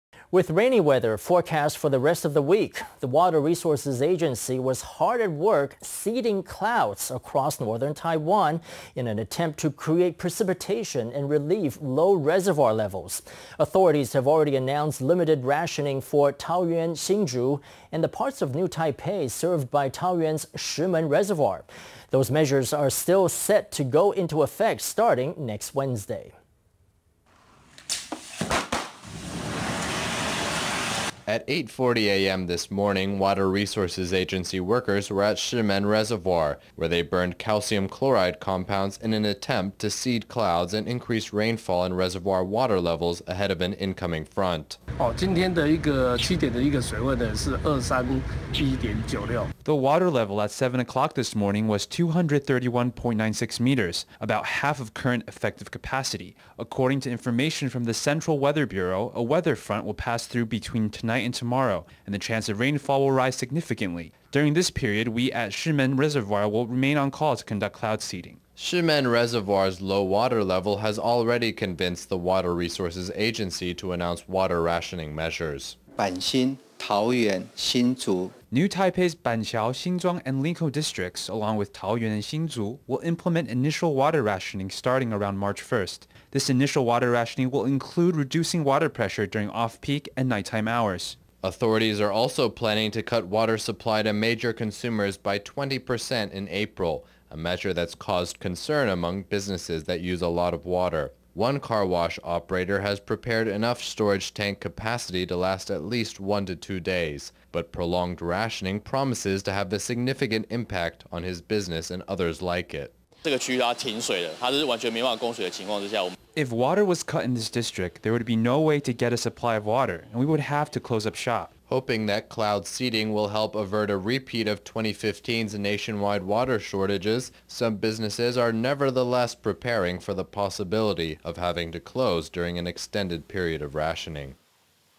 Authorities attempt to relieve water shortage by cloud seeding, as rationing looms on hori...
With rainy weather forecast for the rest of the week ahead, the Water Resources Agency was hard at work seeding clouds across northern Taiwan, in an attempt to relieve low reservoir levels. Authorities have already announced limited rationing for Taoyuan, Hsinchu, and the parts of New Taipei served by Taoyuan’s Shihmen Reservoir. Those measures are still set to go into effect starting next Wednesday. At 8:40 this morning, Water Resources Agency workers were at Shihmen Reservoir, where they burned calcium chloride compounds in an attempt to seed clouds and increase rainfall and reservoir water levels ahead of an incoming front.  Chang Chia-rungShihmen Reservoir DirectorThe water level at seven o’clock this morning was 231.96 meters, about half of current effective capacity. According to information from the Central Weather Bureau, a weather front will pass through between tonight and tomorrow, and the chance of rainfall will rise significantly. During this period we at Shihmen Reservoir will remain on call to conduct cloud seeding.Shihmen Reservoir’s low water level has already convinced the Water Resources Agency to announce water rationing measures.Lai Chien-hsinWater Resources Agency Director-GeneralNew Taipei’s Banqiao, Xinzhuang (and Linkou) Districts, along with Taoyuan and Hsinchu, will implement initial water rationing starting around March 1. This initial water rationing will include reducing water pressure during off-peak and nighttime hours. Authorities are also planning to cut water supply to major consumers by 20 percent in April, a measure that’s caused concern among businesses that use a lot of water. One car wash operator has prepared enough storage tank capacity to last at least one to two days, but prolonged rationing promises to have a significant impact on his business and others like it.Li Chen-yuCar Wash OperatorIf water was cut in this district, there would be no way to get a supply of water, and we would have to close up shop.Hoping that cloud seeding will help avert a repeat of 2015’s nationwide water shortages, some businesses are nevertheless preparing for the possibility of having to close during an extended period of rationing.